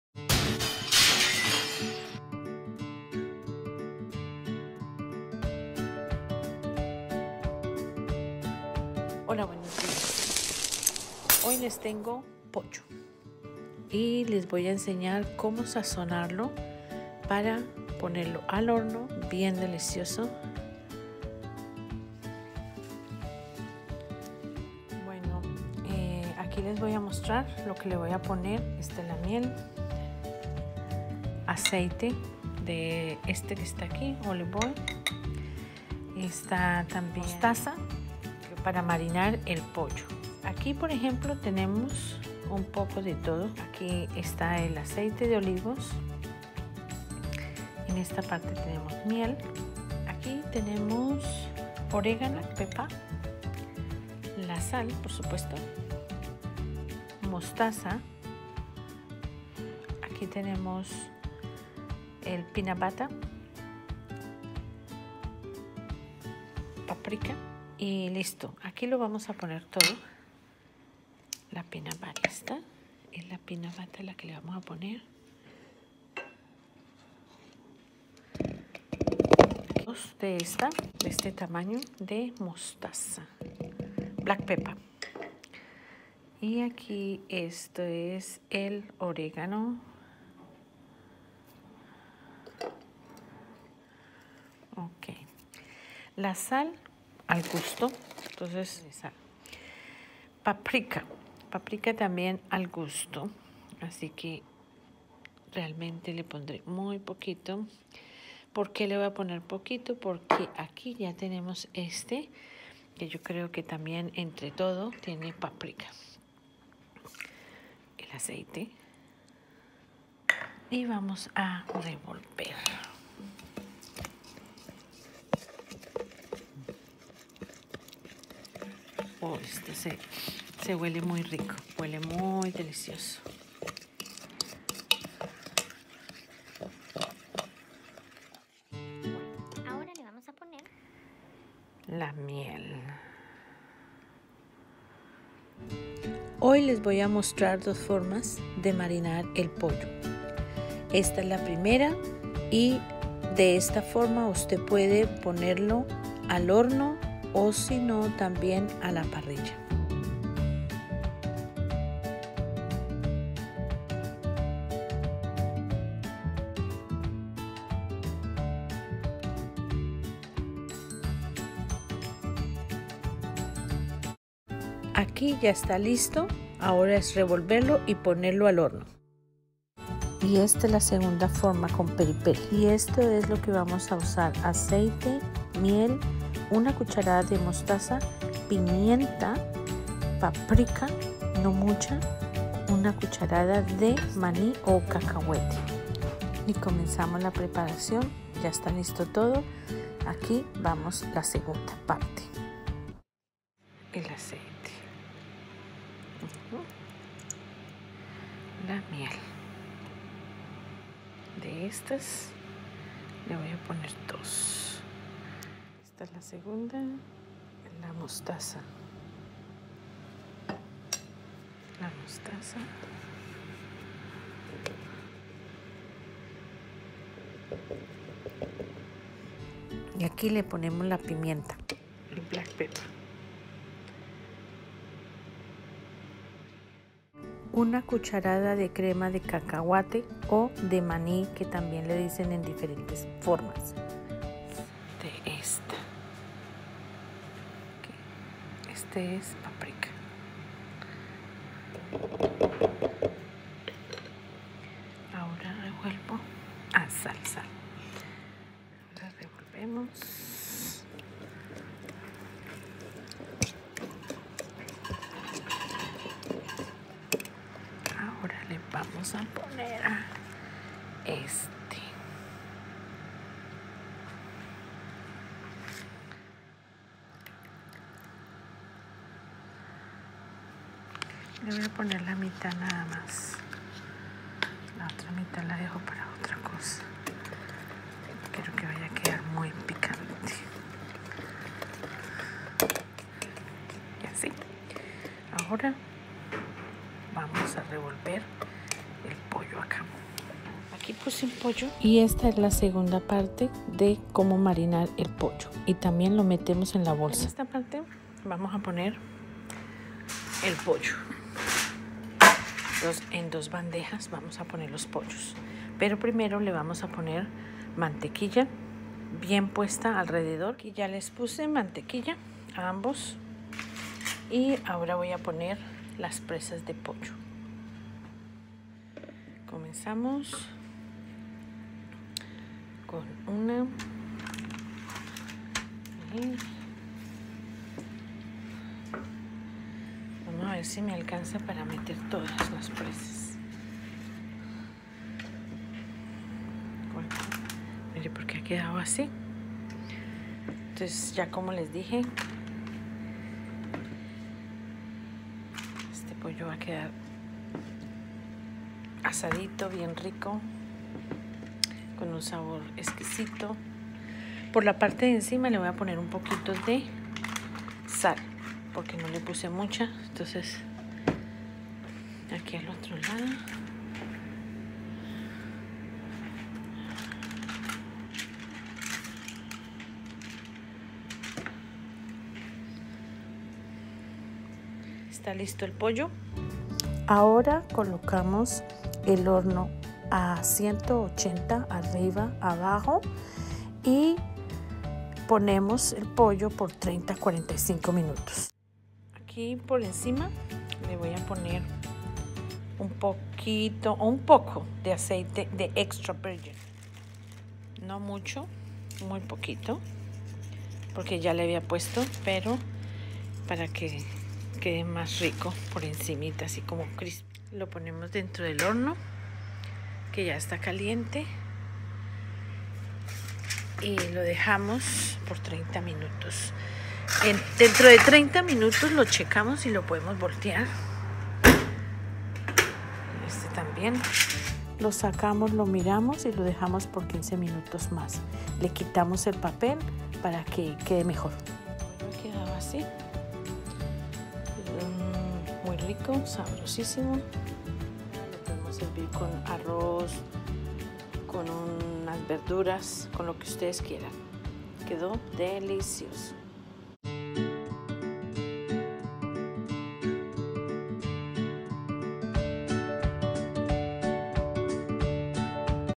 «Pollo al horno, o a la parrilla» 2 maneras de marinarlo, Shaba Cocina delicioso 😋
«Pollo al horno, o a la parrilla»
2 maneras de marinarlo,
Shaba Cocina delicioso 😋

Ingredientes:
10 presas aproximadamente

2 cucharadas de aceite
1 cucharada de miel 🍯
1 cuchara 🥄 de cacahuetes 🥜
1/4 de un paquete de Sansón (diferente hierbas)
1/2 cucharadita de pimienta
1/2 cucharadita de mostaza
Sal al gustó
Paprika al gustó
Mantequilla para la bandeja

Nota:
En la segunda preparación la diferencia es que te cambia a el Peri Peri que es líquido y no solo ponga tampoco todo 1/4 es suficiente para esta cantidad de pollo, si a usted le gusta que quede bien picante póngaselo todo.
Ponerlo al horno a 180– y total por 45 minutos, primero por 30 minutos de hay le quitas el papel y lo dejas 15 minutos más, para que quede tostado por encima.

2 cucharadas de aceite
1 cucharada de miel 🍯
1 cuchara 🥄 de cacahuetes 🥜
1/4 de un paquete de peri peri,Sansón para pollo 🐓
1/2 cucharadita de pimienta
1/2 cucharadita de mostaza
Sal al gustó
Paprika al gustó
Mantequilla para ponerle a la bandeja